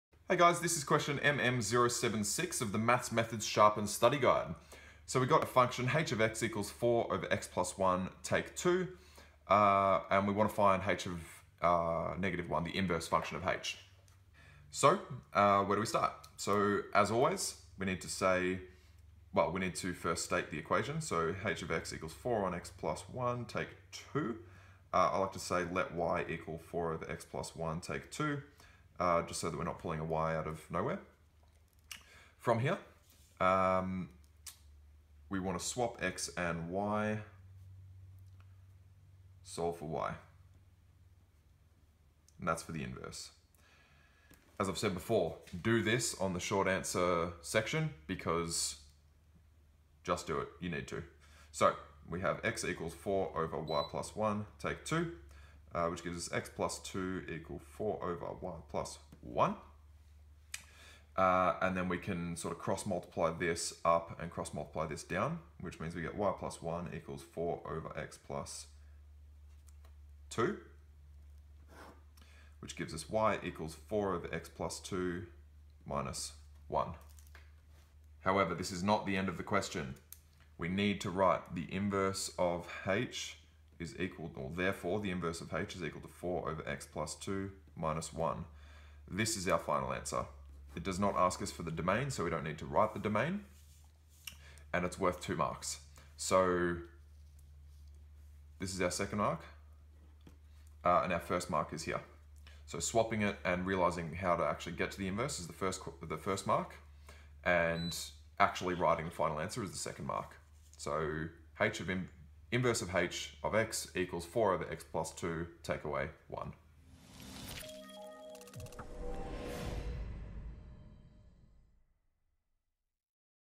MM076 Maths Methods Fortify Study Guide - "How to find the inverse of a hyperbola"
This online video solution has been created specifically for the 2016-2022 FORTIFY: VCE Mathematical Methods Units 3/4 Study Guide.